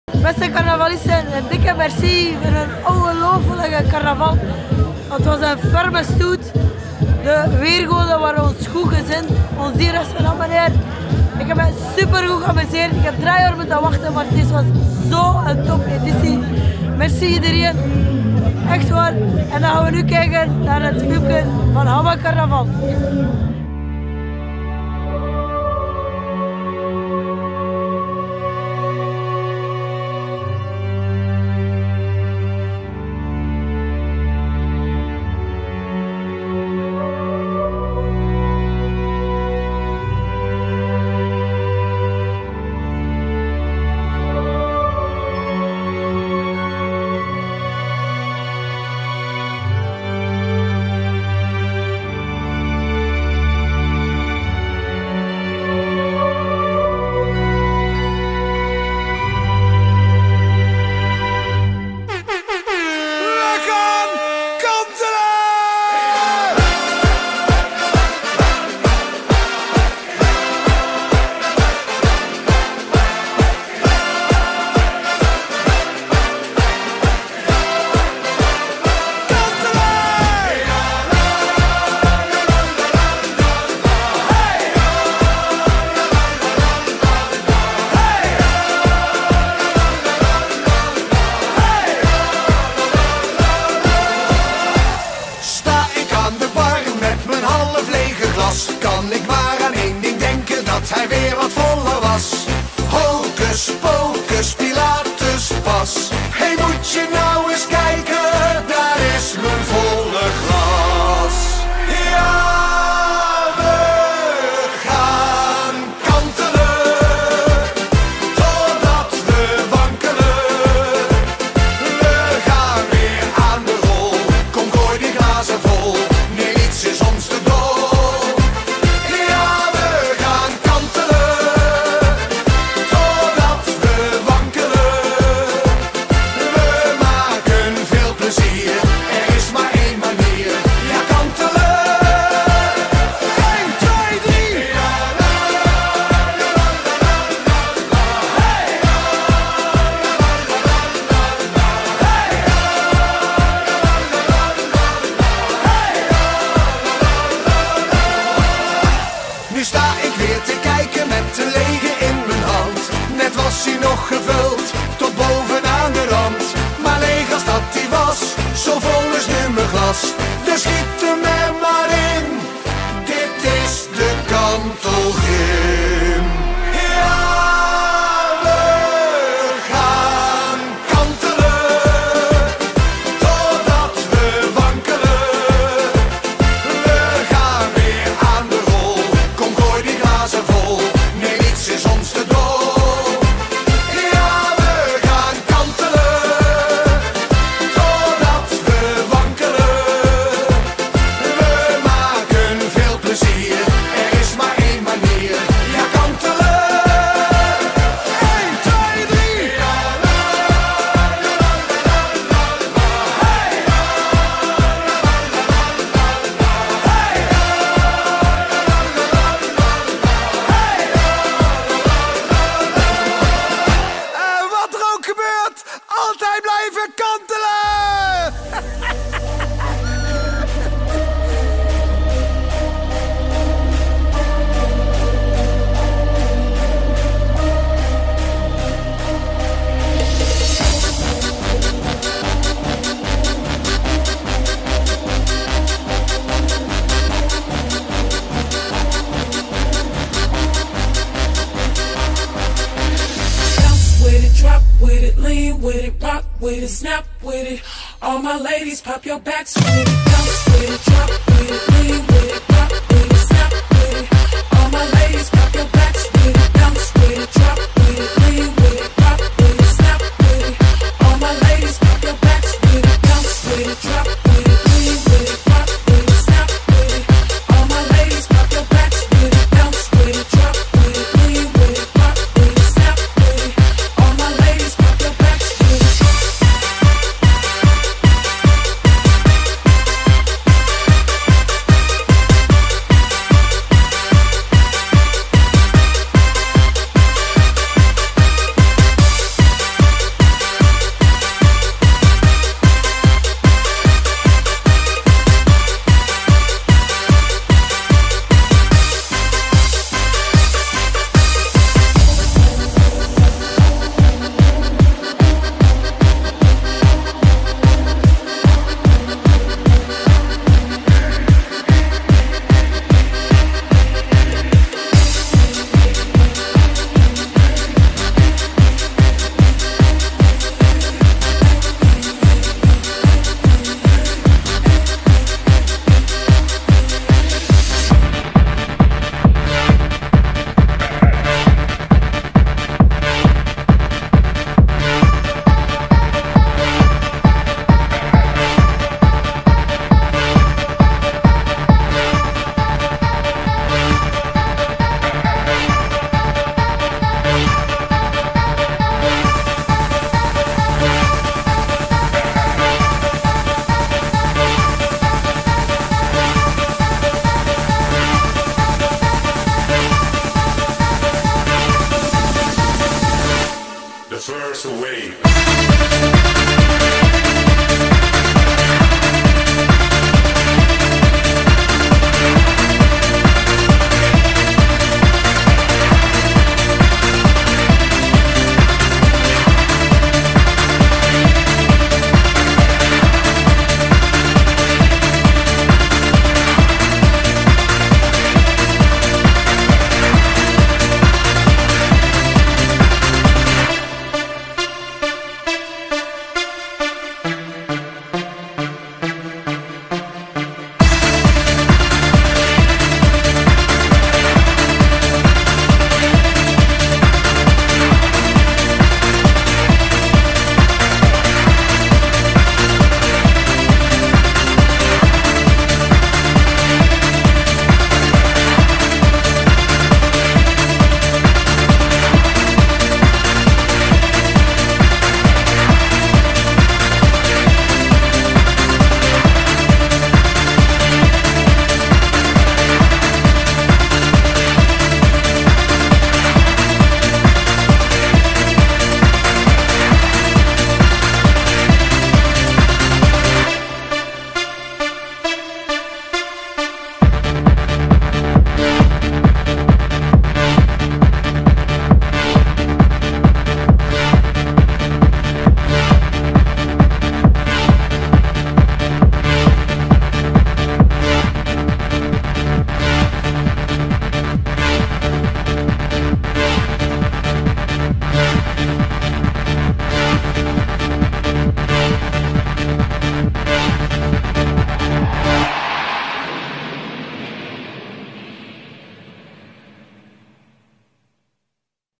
Aftermovie Hamme Carnaval 2023
na 3 jaren wachten mocht prinses Ananas eindelijk vieren met Hamme carnaval